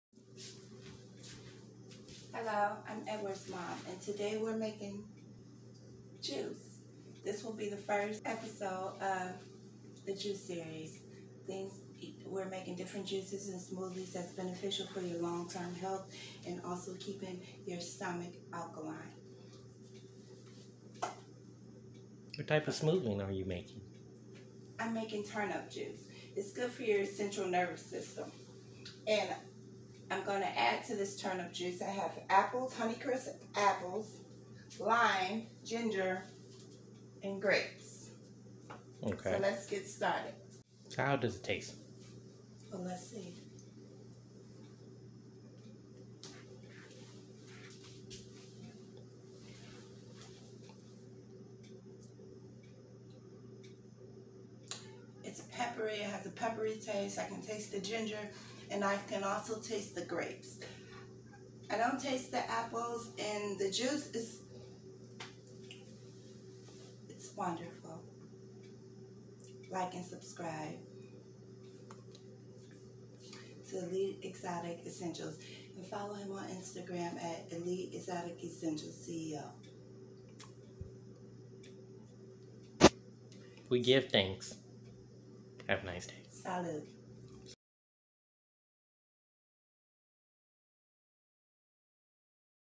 The Juice Series: Episode 1
In this series we will be Showcasing the best juices and smooths for long term health. Turnip juice.